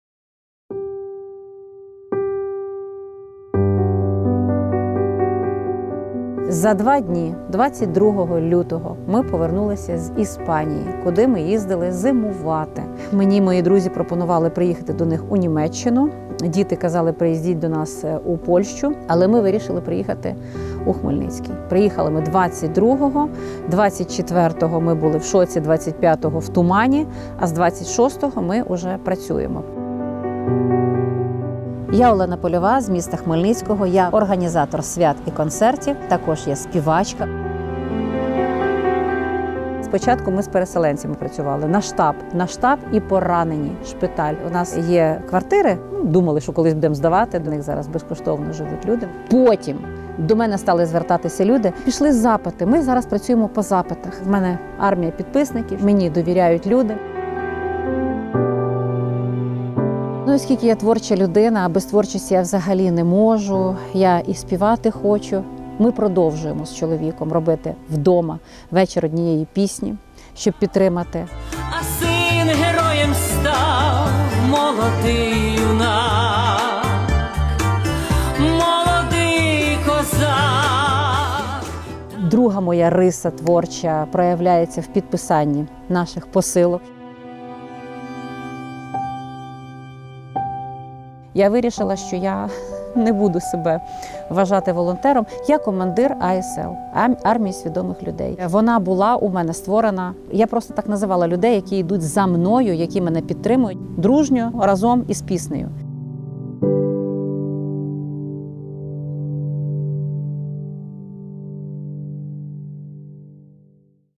Блогерка, ведуча урочистих подій Олена Польова | Мистецький стрій
Олена Польова – блогерка, ведуча та співачка раніше займалася святами, а тепер війною. Під час війни з російським окупантом відкрила штаб допомоги у під’їзді багатоповерхівки, де мешкає. Разом з чоловіком , звукорежисером Олегом Фіщуком, допомагають українським бійцям, переселенцям та мешканцям територій, де йдуть бойові дії. Сьогодні Олена намагається поєднувати творчість з гуманітарною допомогою.